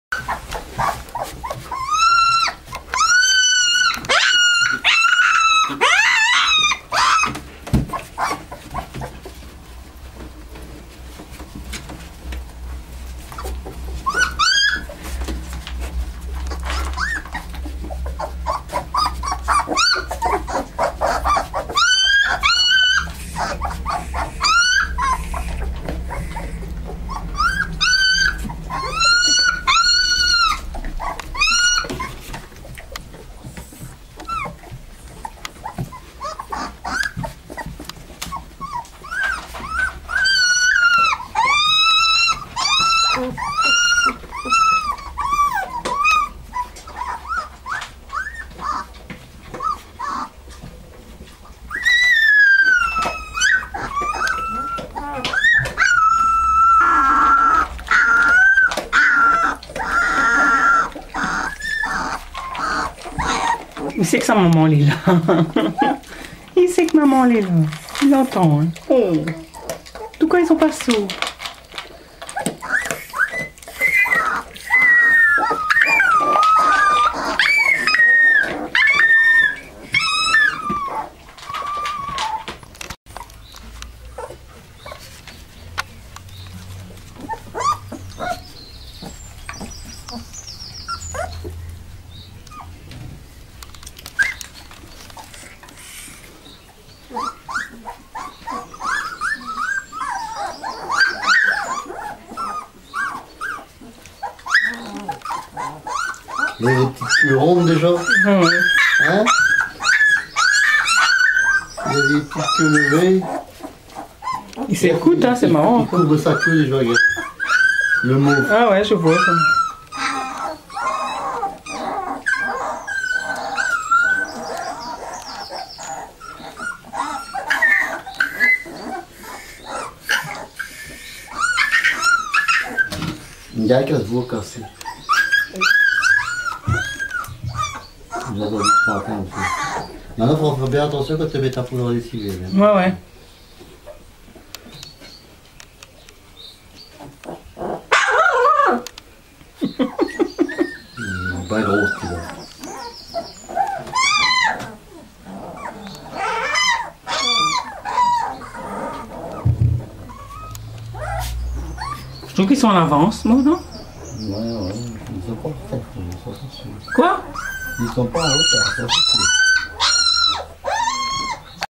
VAL DES HURLUS - Malamute d'Alaska - 23 et 26 juin.avi
les chiots commencent  à bien bouger